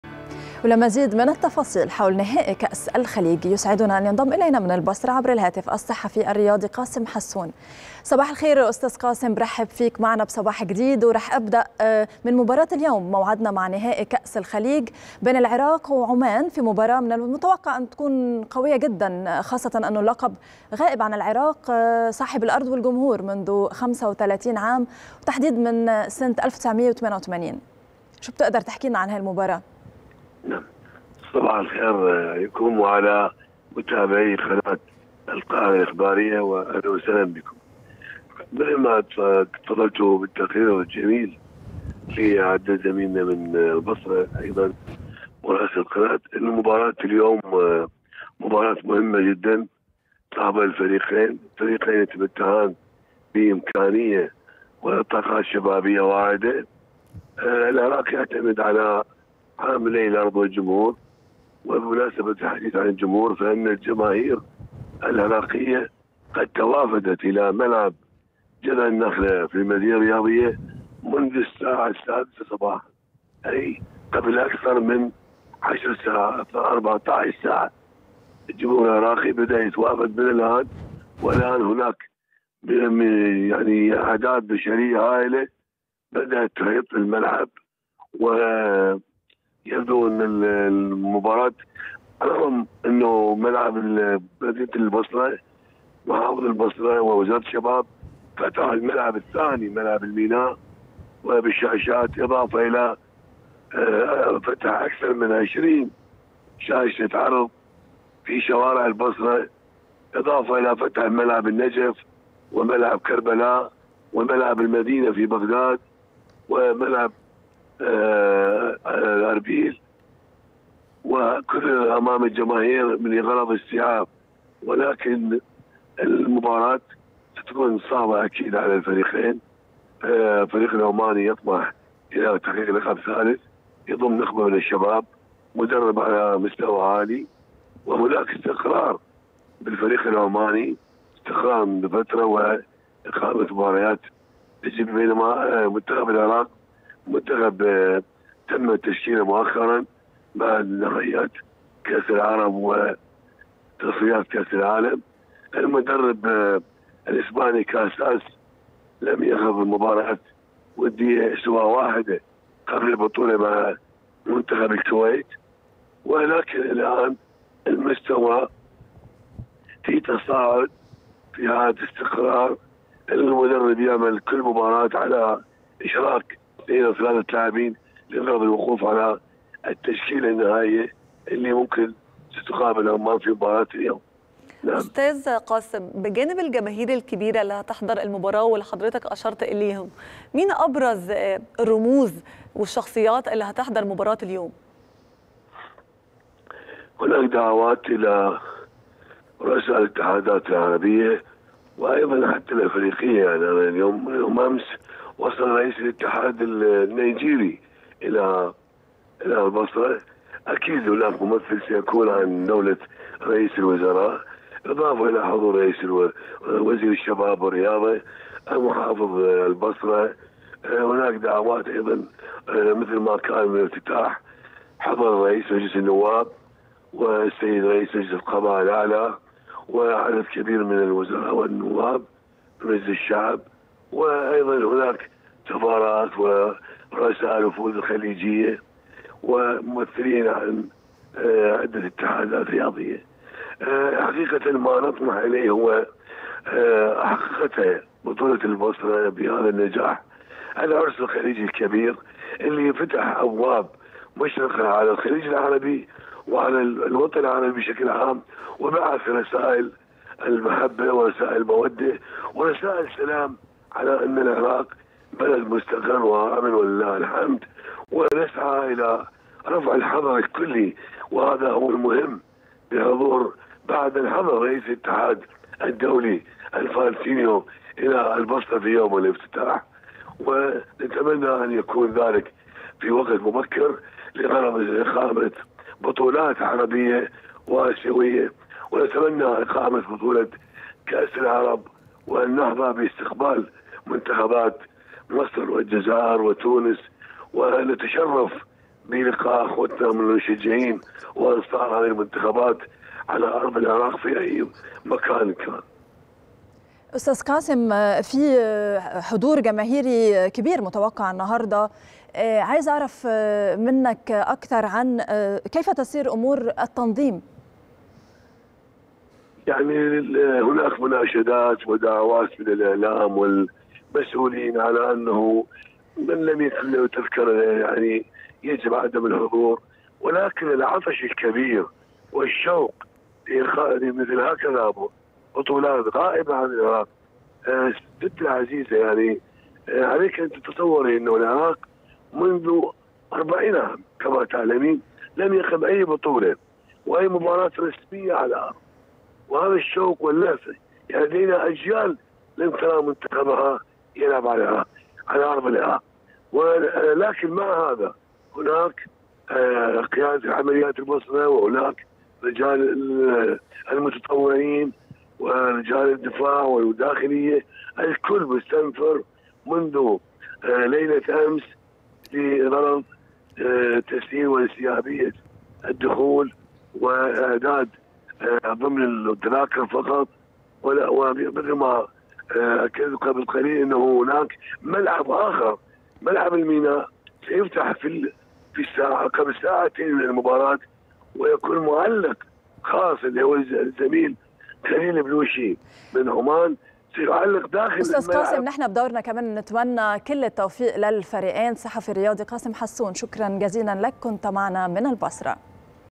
العراق يبحث عن اللقب الغائب منذ 35 عاما.. وعُمان تسعى لتحقيق اللقب التاريخي
العراق يبحث عن اللقب الغائب منذ 35 عاما.. وعُمان تسعى لتحقيق اللقب التاريخي | التفاصيل مع قاسم حسون صحفي رياضي عراقي

19/1/2023